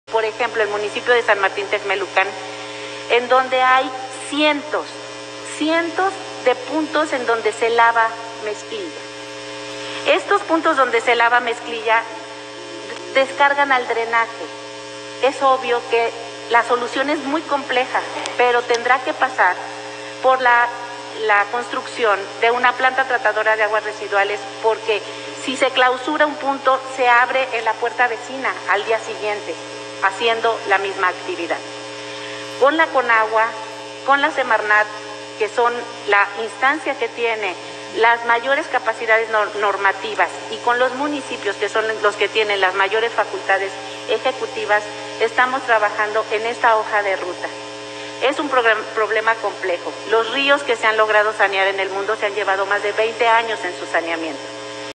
Medio Ambiente de Puebla no ha clausurado los cientos de puntos de contaminación del río Atoyac
La Secretaría de Medio Ambiente de Puebla no ha clausurado los cientos de puntos de contaminación del río Atoyac detectados en San Martín Texmelucan, porque planea la construcción de una planta tratadora de aguas residuales, informa su titular Beatriz Manrique